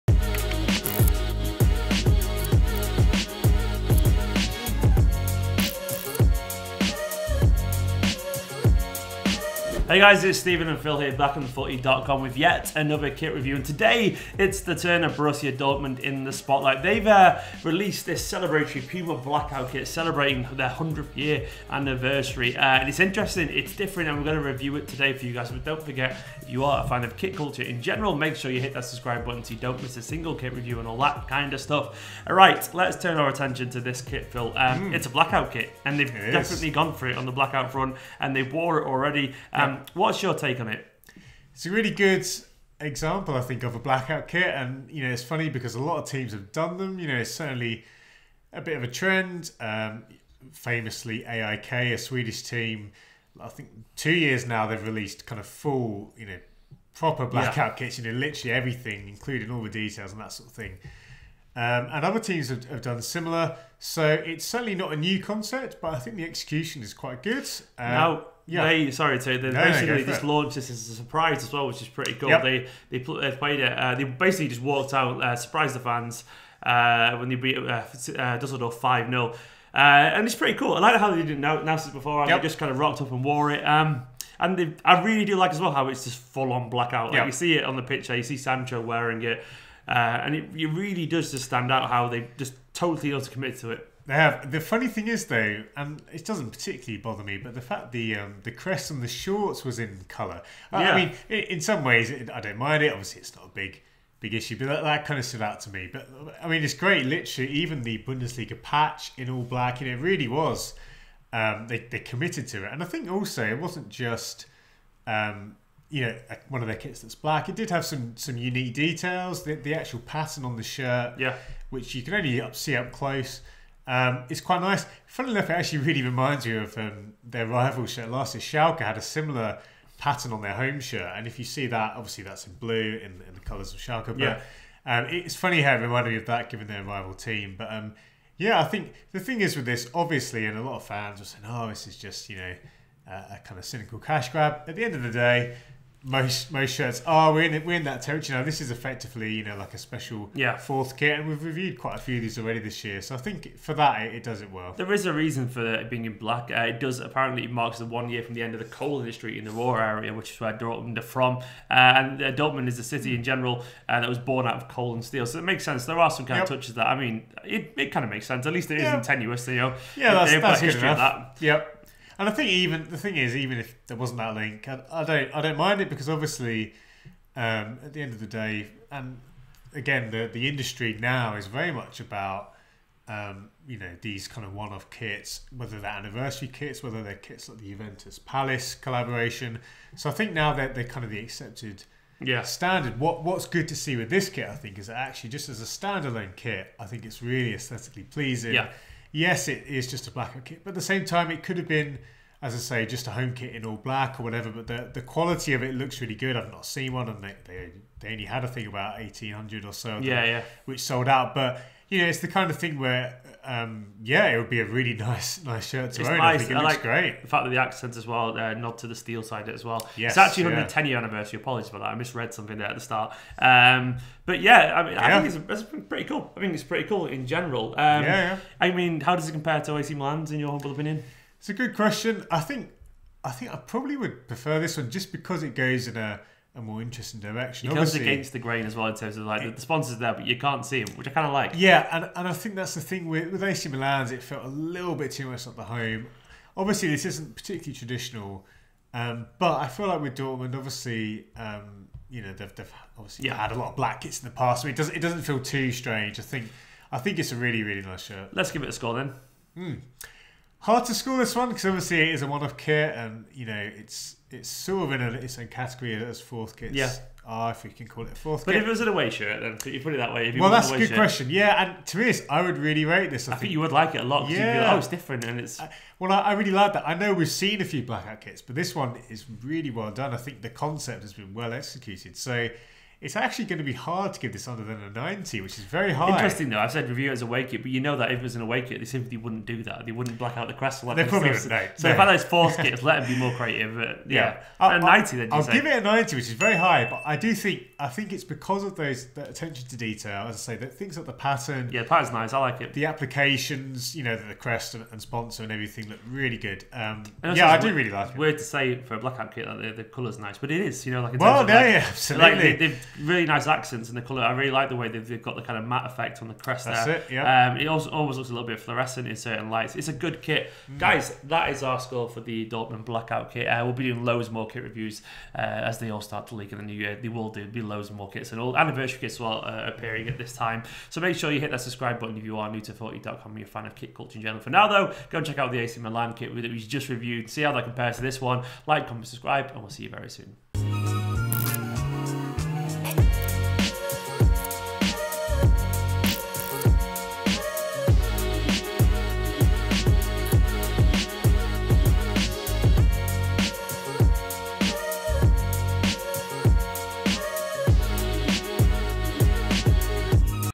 BORUSSIA DORTMUND 110TH ANNIVERSARY BLACKOUT PUMA SHIRT | KIT REVIEW
Borussia Dortmund launch their stunning 110th anniversary jersey from PUMA. The blackout jersey was dropped unexpectedly in a 5-0 win over Dusseldorf at Signal Iduna Park, and it's gorgeous. We review the kit.
⚽️ Compare prices for all football gear!
👕 Boots, kits, match balls and more...

The special edition kit, which took fans by surprise ahead of kick off coincidently also caught the club by surprise as it sold out in a matter of minutes, helped no doubt by braces from Marco Reus and Jadon Sancho and another goal from Thorgan Hazard. The unexpected drop even crashed to BVB official online store at one point, such was the demand.

One year on from the end of the coal industry in the Ruhr area, Borussia Dortmund marked the moment in a black shirt, shorts and socks, which included the logos of all partners: Evonik, PUMA, Opel and the Bundesliga. After all, BVB and the city of Dortmund were born out of coal and steel.